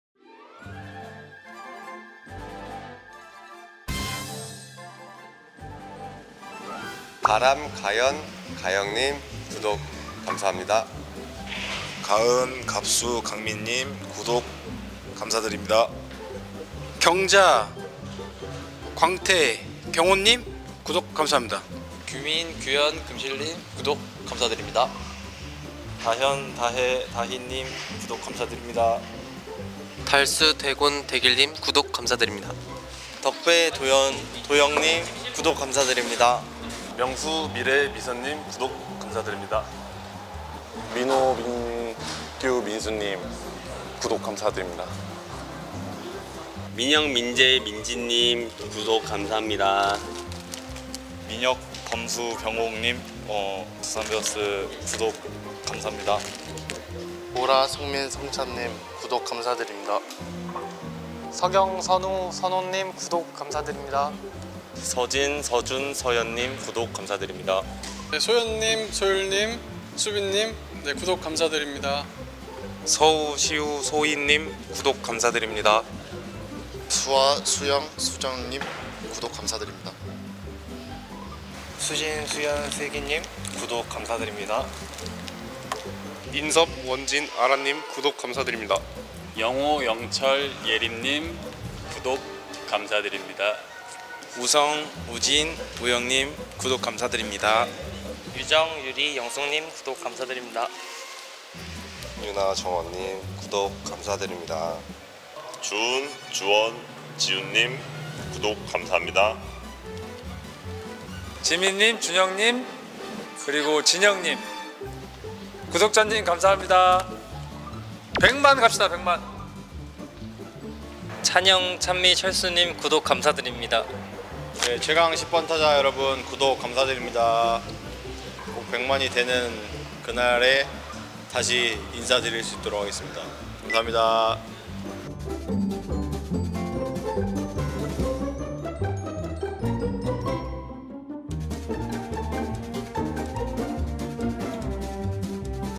🎊BEARS TV 구독자 20만 돌파🎊 조심스럽게 불러보는 우리 최강10번타자들의 이름..😮🎤
감사하고.. 또 감사합니다 💙🐻💙

지금까지 보내주신 최강10번타자 여러분들의 관심과 사랑에 보답하기 위해! 이벤트를 준비했습니다 🎁
이벤트에 대한 상세 내용은 🔥고정 댓글🔥을 통해 확인해 주세요! 😁

[타임라인]
00:07 가람, 가연, 가영
00:12 가은, 갑수, 강민
00:17 경자, 광태, 경호
00:22 규민, 규현, 금실
00:26 다현, 다혜, 다희
00:30 달수, 대곤, 대길
00:33 덕배, 도연, 도영
00:37 명수, 미래, 미선
00:41 민호, 민규, 민수
00:47 민영, 민재, 민지
00:51 민혁, 범수, 병옥
00:57 보라, 성민, 성찬
01:01 서경, 선우, 선호
01:05 서진, 서준, 서현
01:08 소연, 소율, 수빈
01:13 서우, 시우, 소희
01:17 수아, 수영, 수정
01:22 수진, 수현, 슬기
01:26 인섭, 원진, 아라
01:29 영호, 영철, 예림
01:34 우성, 우진, 우영
01:38 유정, 유리, 영숙
01:42 윤아, 정원
01:45 주은, 주원, 지훈
01:51 지민, 준영, 진영
02:01 찬영, 찬미, 철수
02:05 최강10번타자